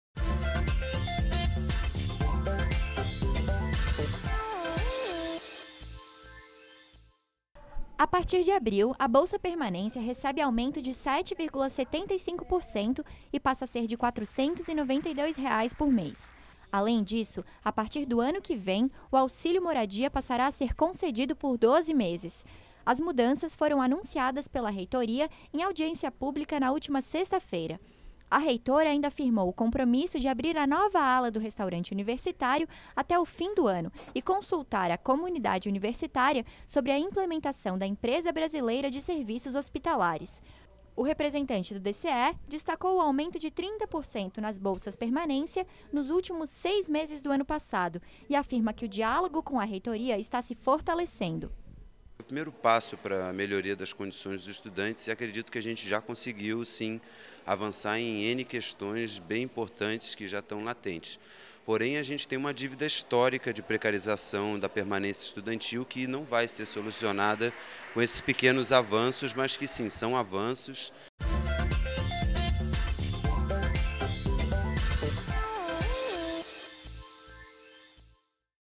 Universidade Já - Audiência Pública
Foram anunciadas mudanças no período de pagamento da bolsa-permanência e outras medidas de apoio ao estudante. Participaram da audiência representantes da UFSC e do Diretório Central dos Estudantes.